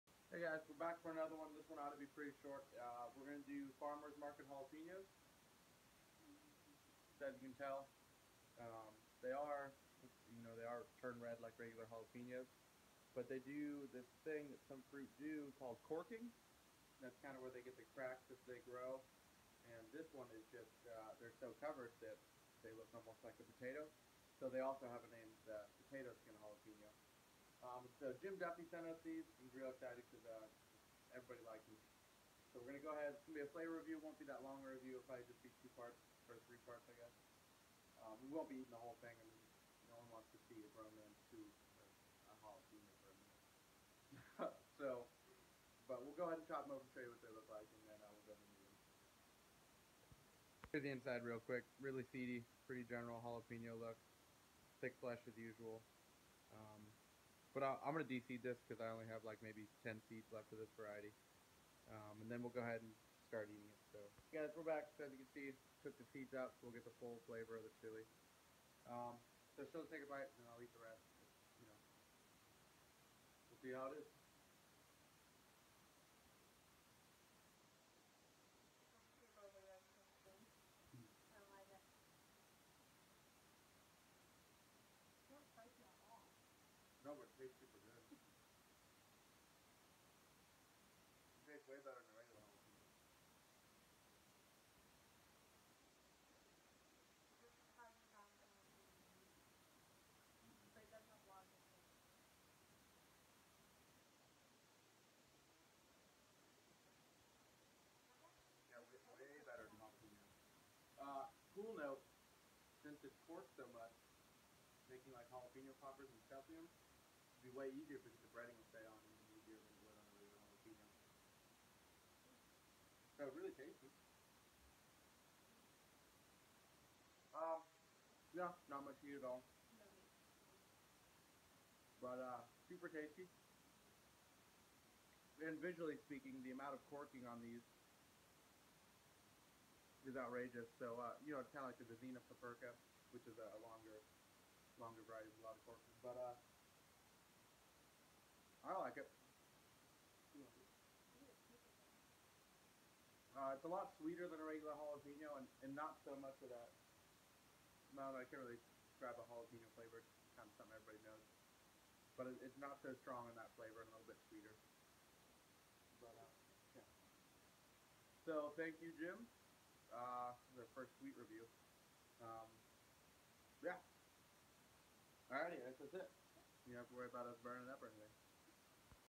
Farmer's Market Jalapeño Pod Review
This is a much Milder, More sweet and less floral Jalapeño variety that needs to start being sold everywhere! It is wonderful! Seeds can be found at Refiningfirechiles.com from Jim Duffy!